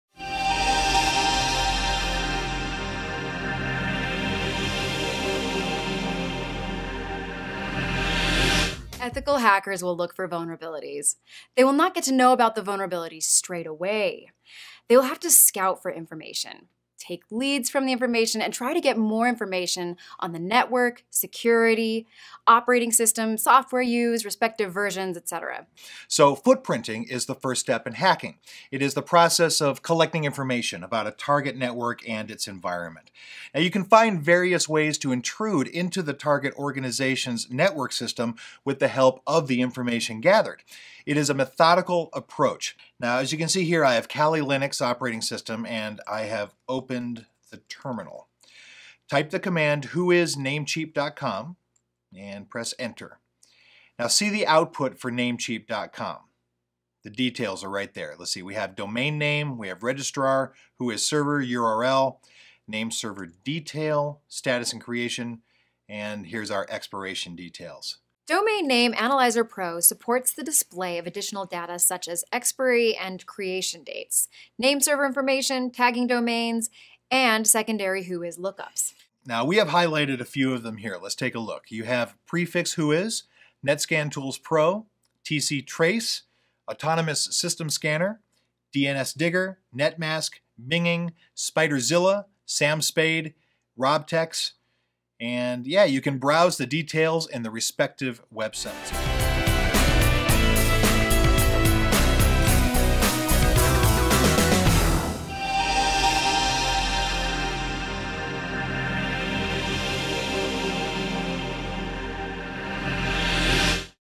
Course Promo: Footprinting
Ethical hackers will look for vulnerabilities. They will have to scout for information, take the leads form the information, and try to get more information on the network, security, operating system, software used, respective versions, etc.  Footprinting is the process of collecting information about a target network and its environment. In our course, Footprinting, you will be learning about the 7 steps to information gathering, the footprinting hierarchy, and footprinting methodology, as well as the pros and cons of both passive and active footprinting. Google can be used for hacking. Learn the various tools, steps, and components that are used when hacking with Google. Learn the different tools and techniques used for footprinting both active and passive. We will conclude this course by going over various steps to countermeasure footprinting, as well as steps used in footprinting pentesting. View various demos demonstrating several of the tools that are talked about in this course.